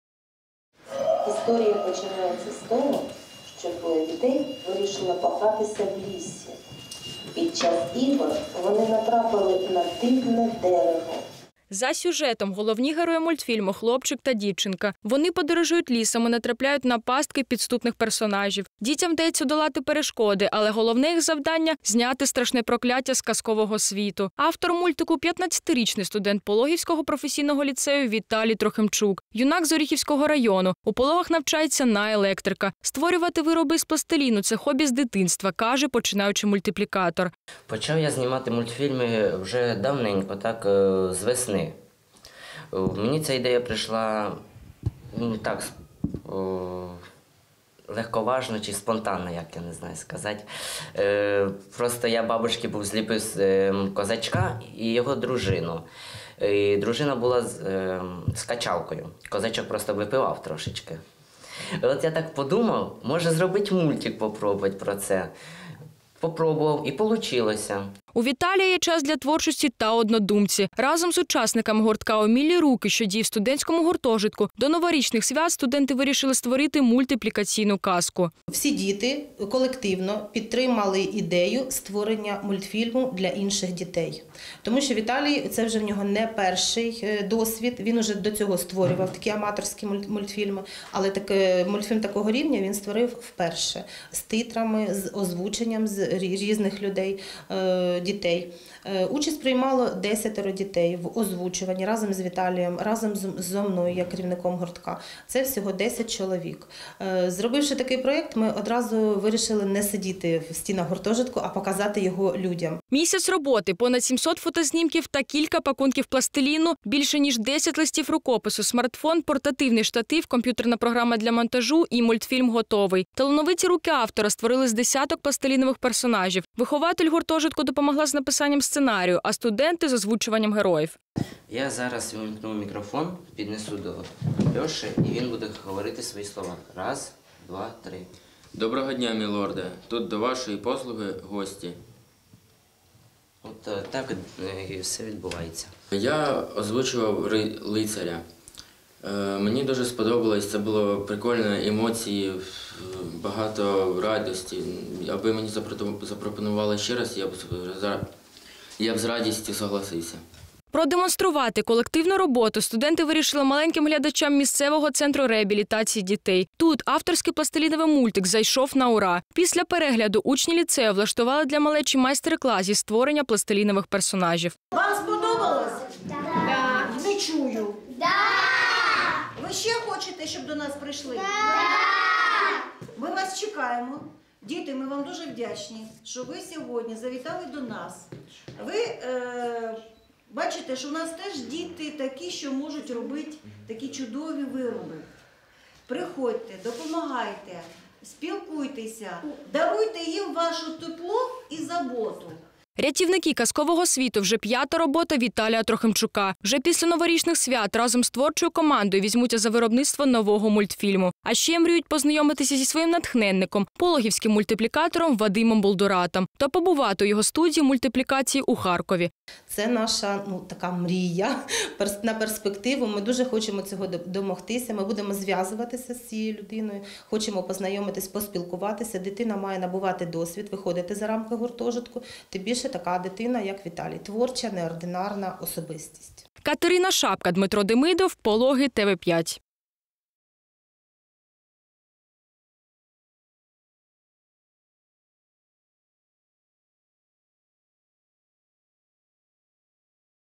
Юнак із Запорізької області створив фантастичну анімовану історію з пластиліну
15-річний Віталій вчиться на електрика, а у вільний час створює мультфільми. Нещодавно він презентував свою нову роботу.